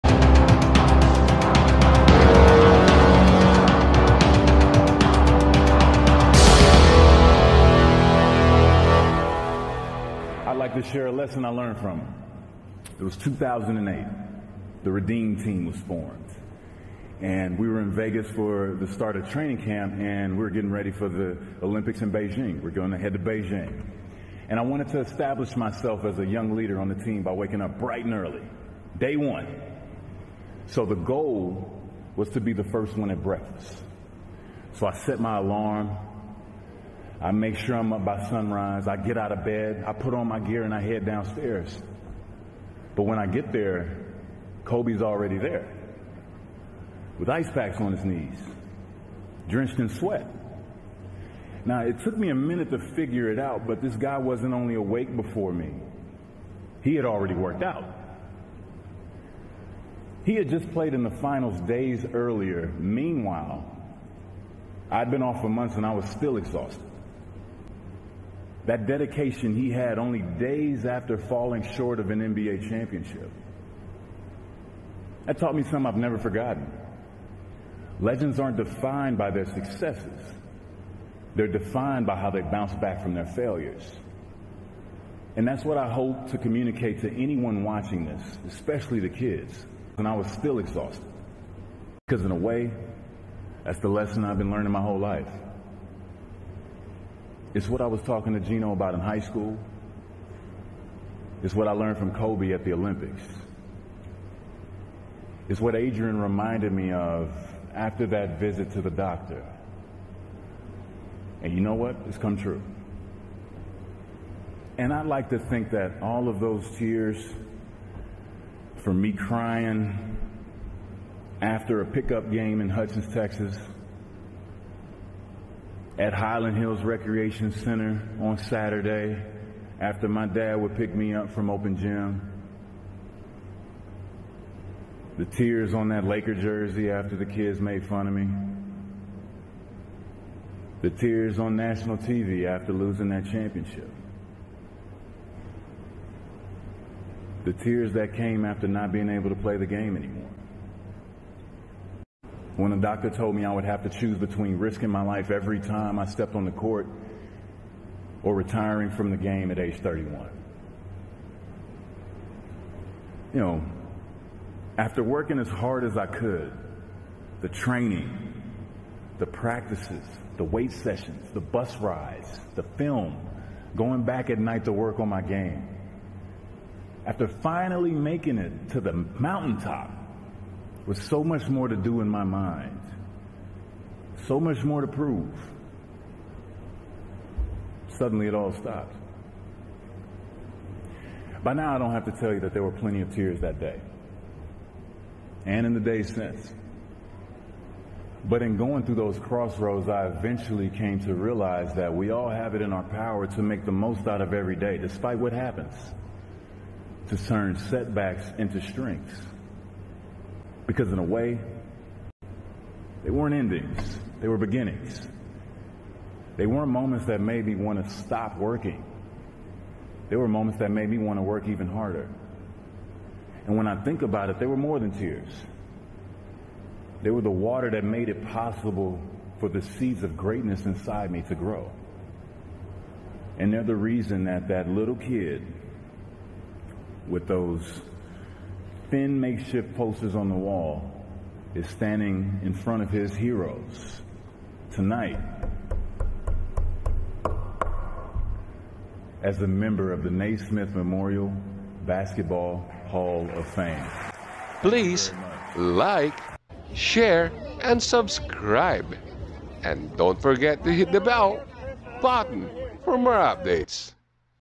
He has to choose from PLAYING or RETIRING at age 31...😢😢 from Chris Bosh's HOF speech...
Hello mga Ka-Pos Life!
This channel is intended to inspire and uplift one's positivity in life